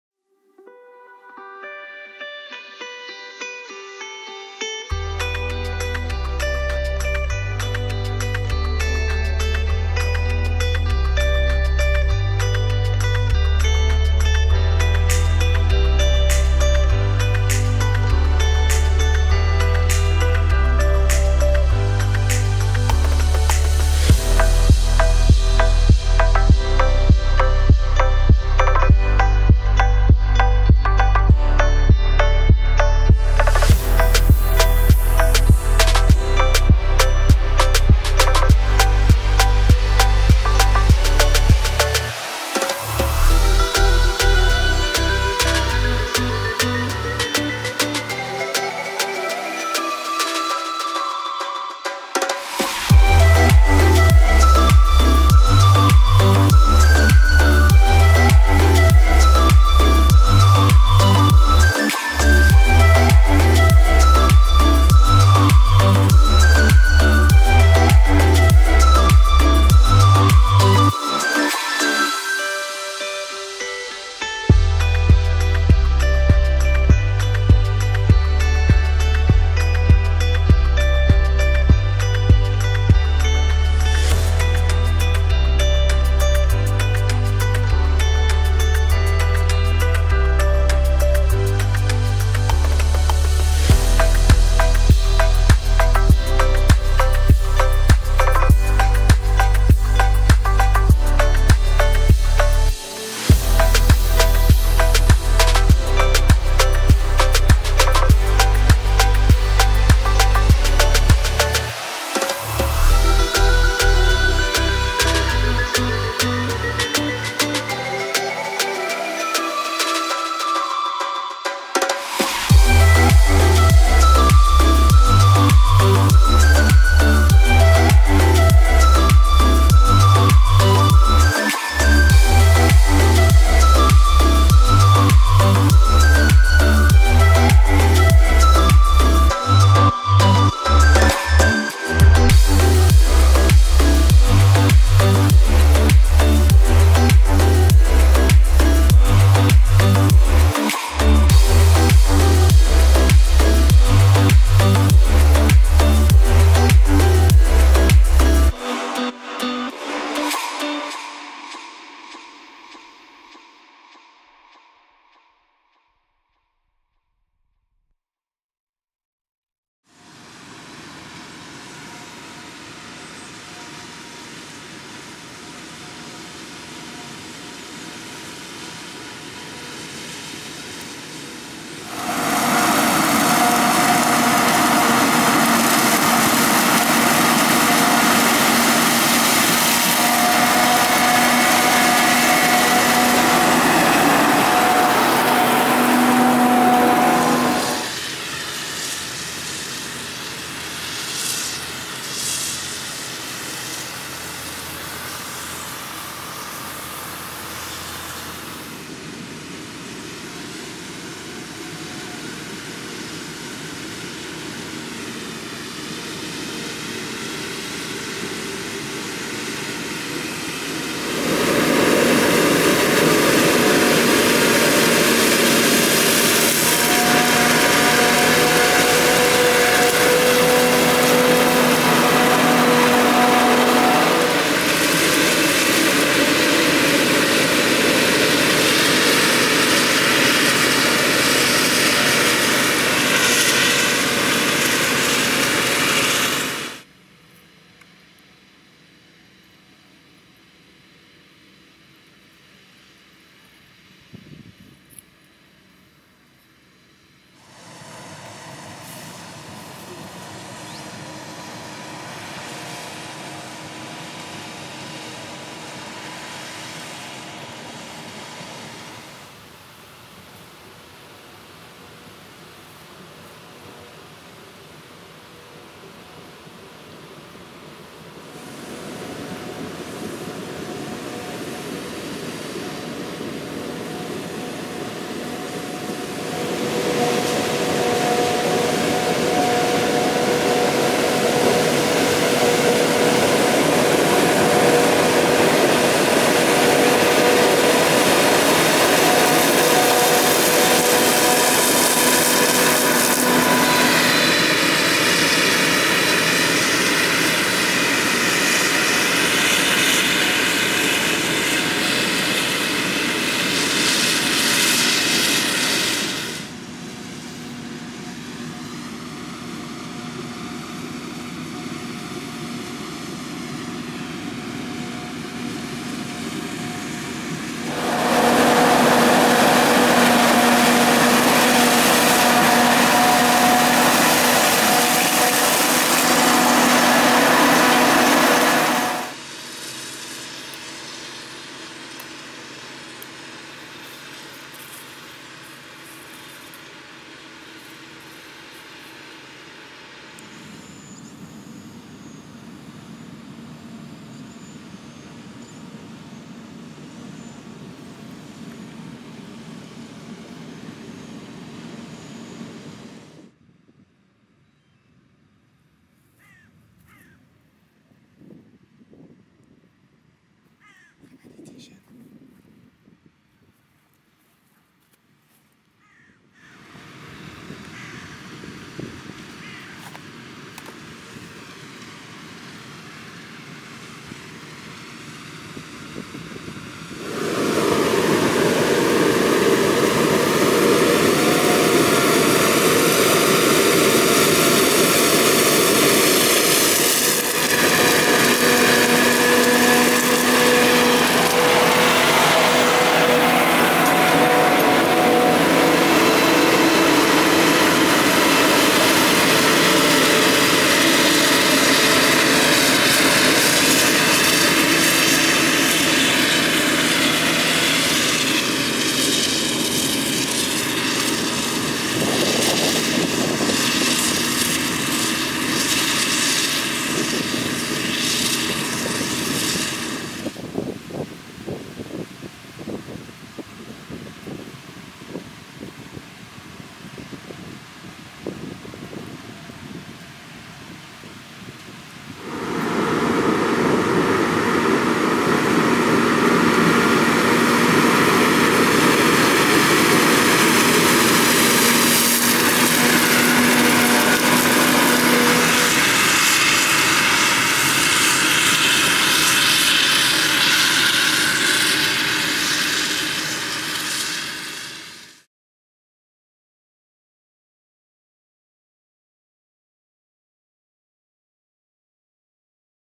2019 | MKS hakselen | Westerveld & Seegers | Claas | Massey Ferguson | John Deere.
► Werkzaamheden: Mks hakselen.
► Loonbedrijf: Westerveld & Seegers.
► Machines: Claas Jaguar | John Deere | Massey Ferguson | Joskin.
► Plaats: Angerlo, Gelderland.
► Land: Nederland | Holland | The Netherlands.